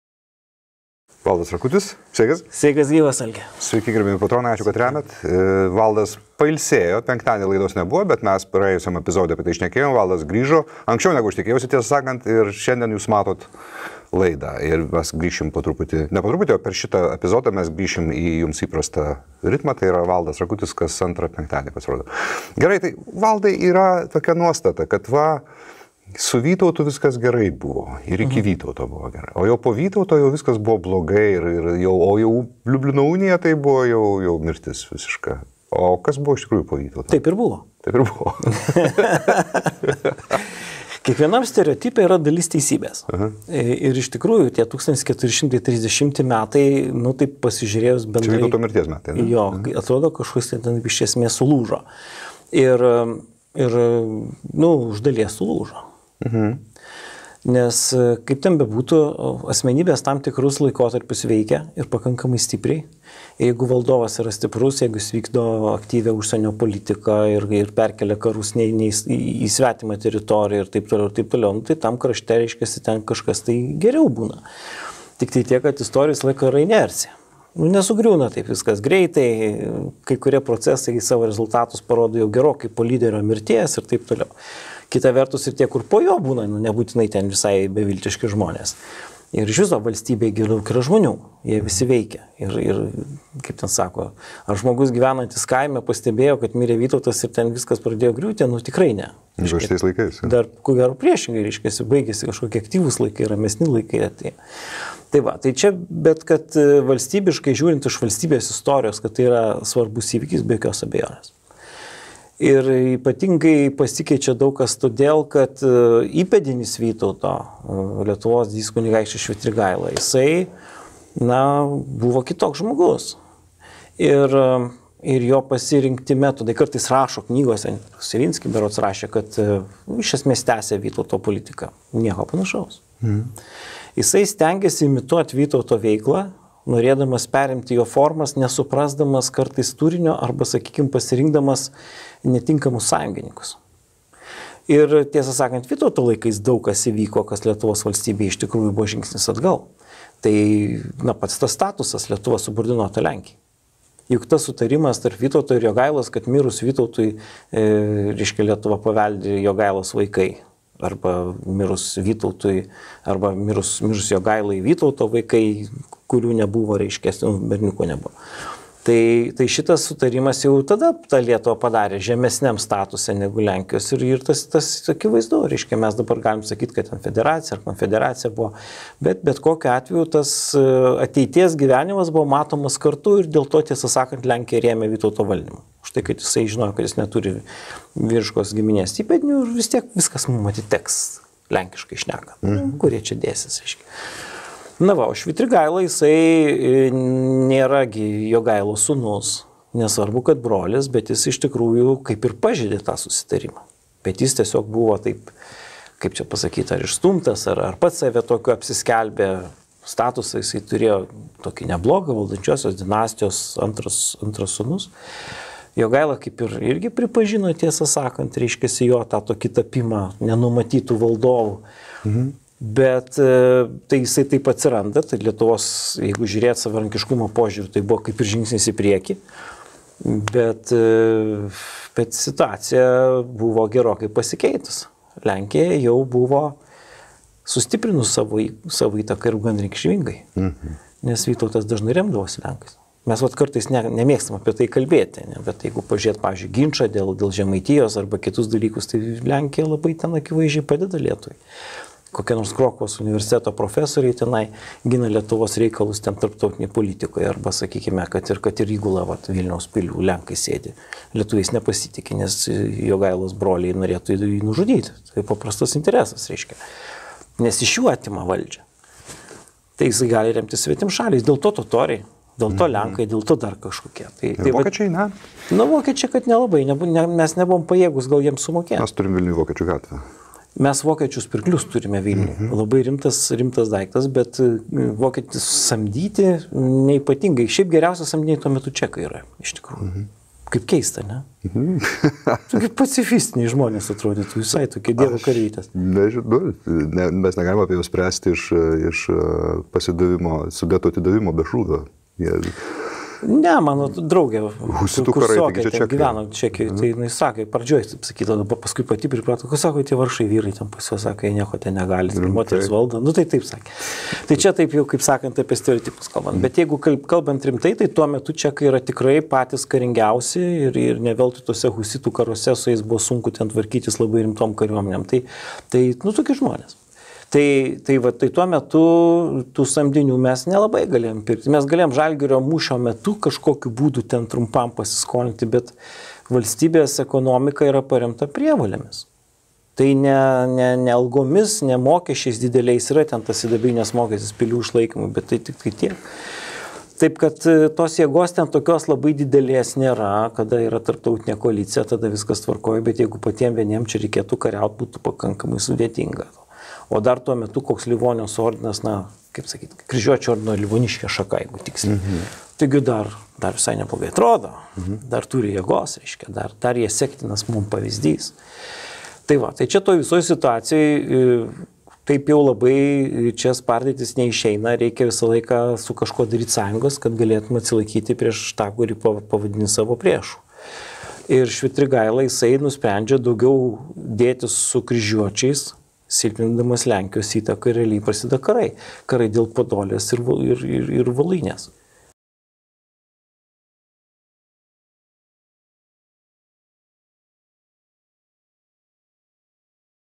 B&R Pristato: prof. Valdas Rakutis: 20201110 LDK Po Vytauto XV a. IŠTRAUKA
Pilietinis karas LDK po Vytauto mirties. Švitrigaila. Kazimieras.